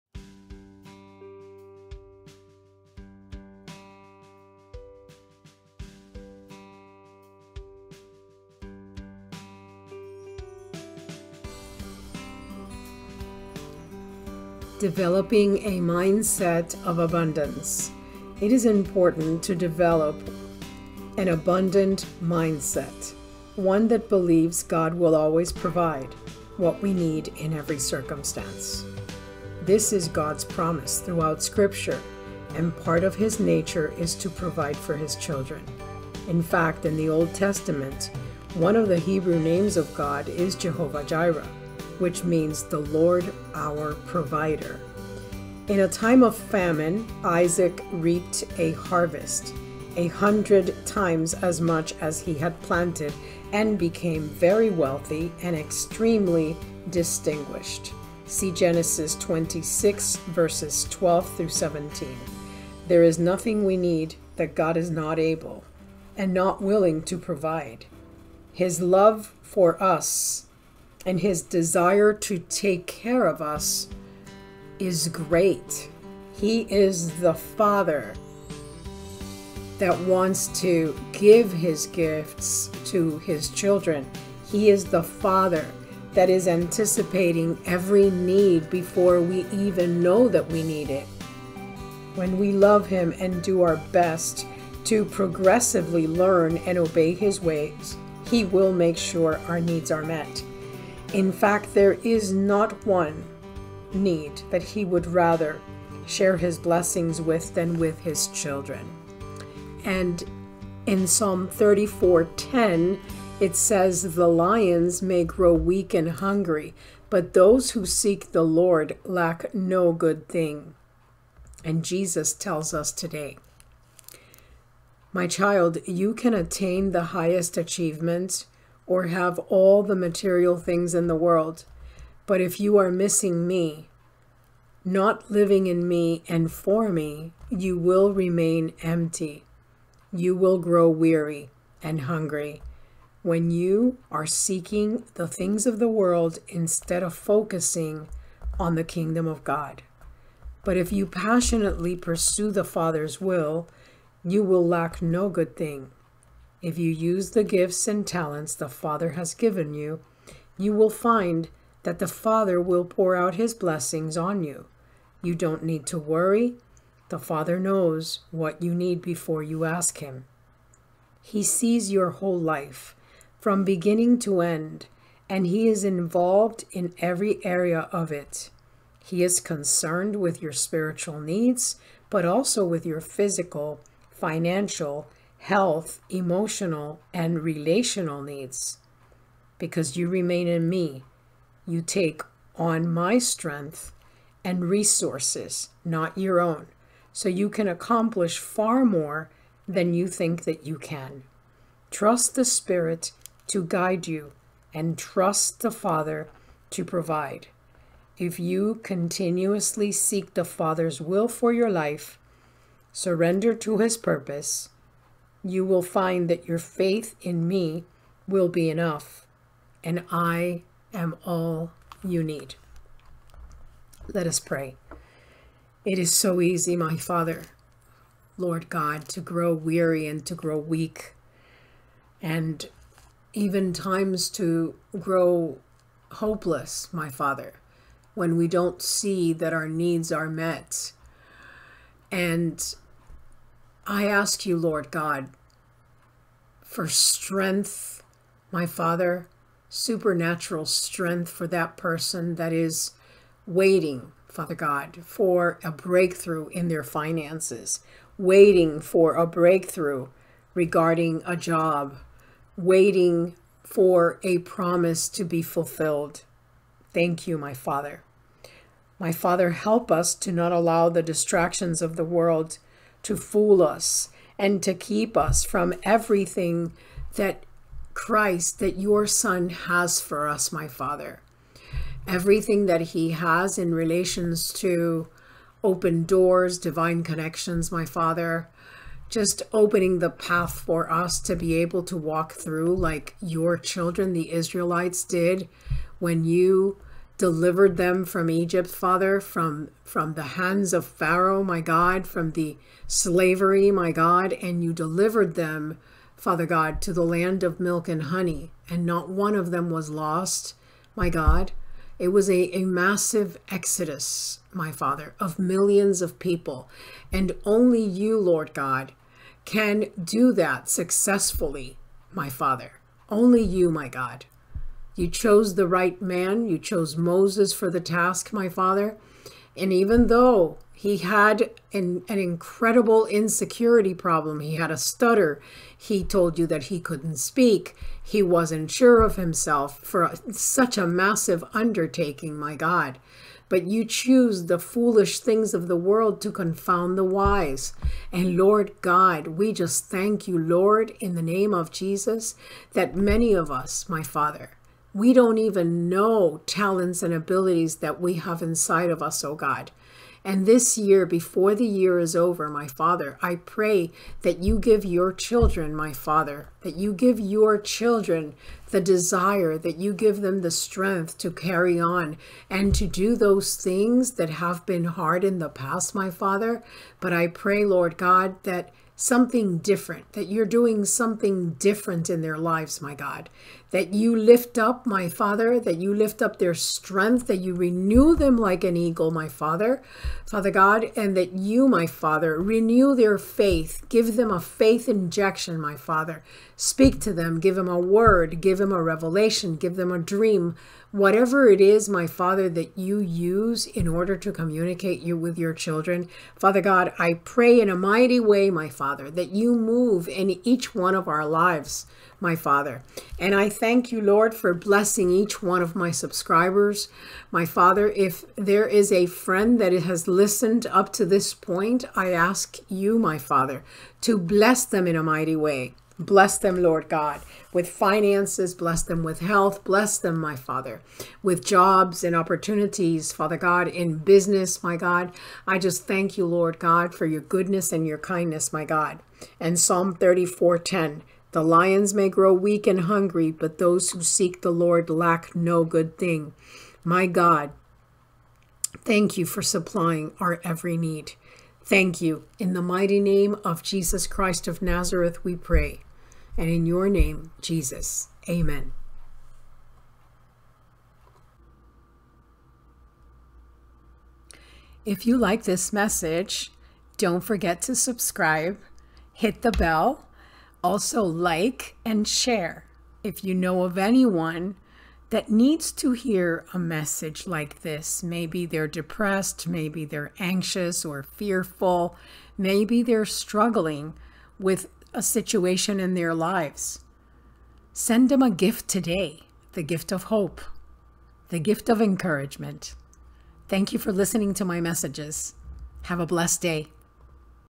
Daily devotion Developing an abundance mindset
Hello friend,
An abundant mindset starts by believing God can and he will provide for all your needs.
Blessings,
Gloria